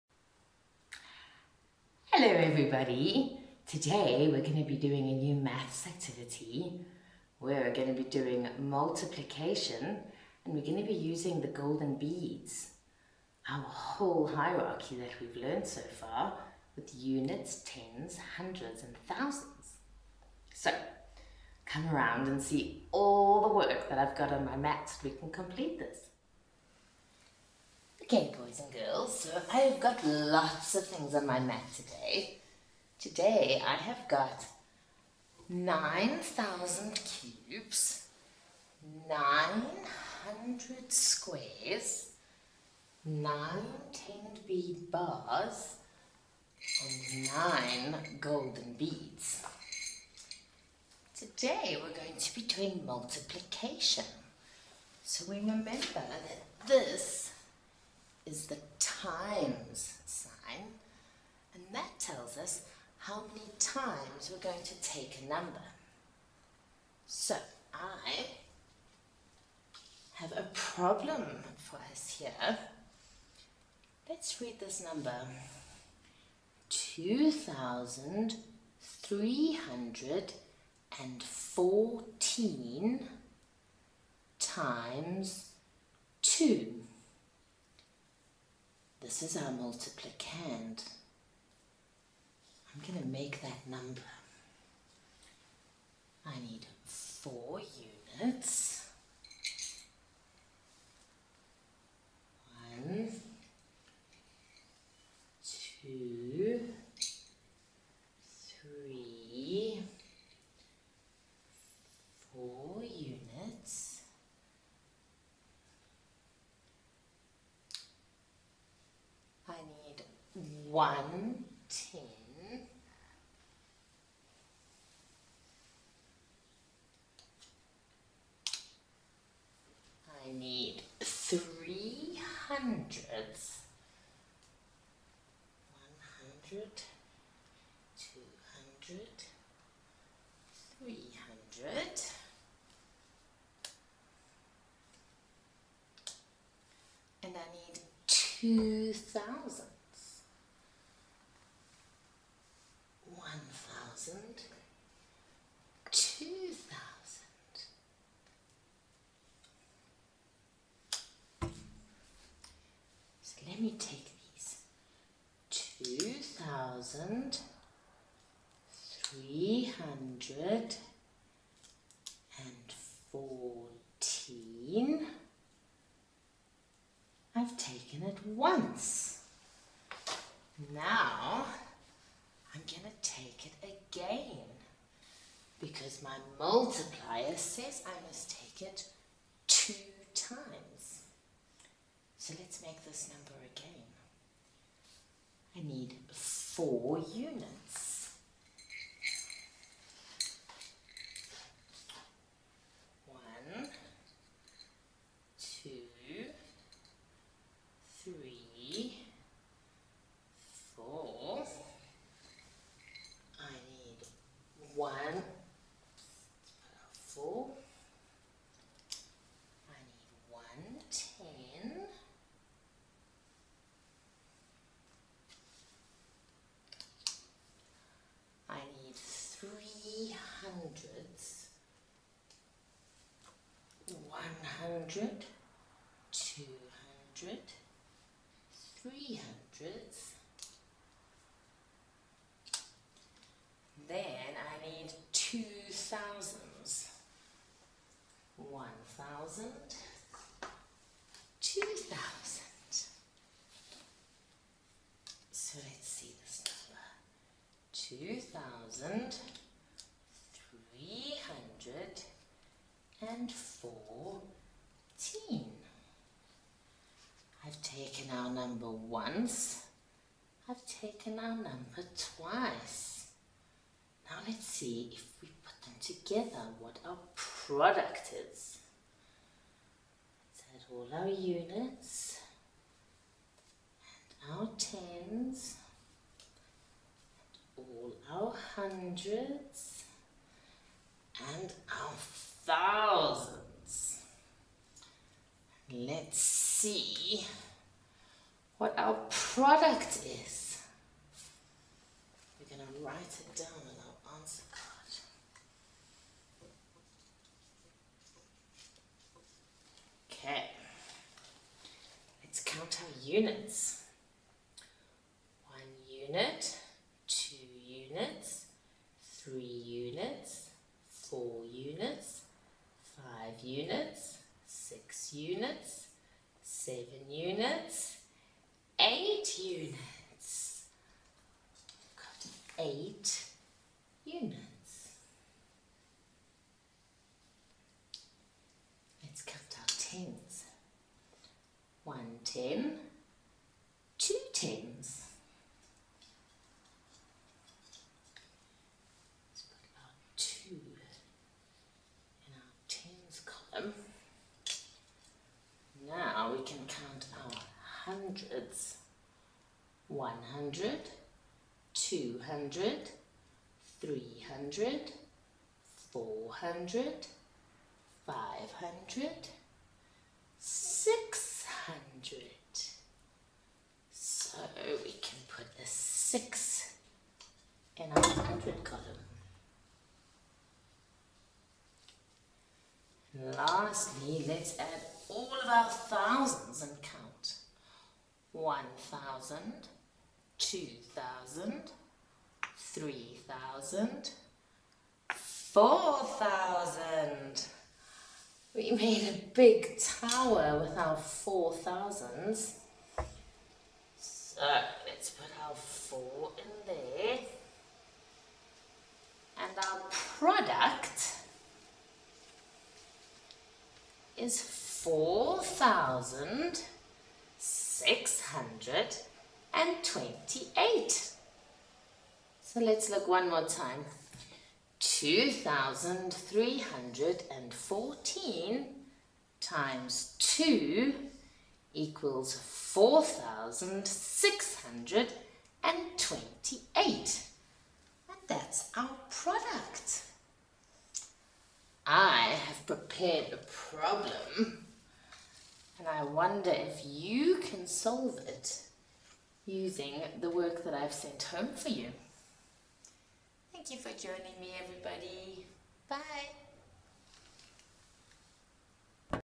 Montessori Multiplication with the Golden Beads units, tens, hundreds and thousands
This video introduces the child to multiplying large numbers using the golden beads and small number cards. This is the most concrete representation of multiplication in the hierarchy of number.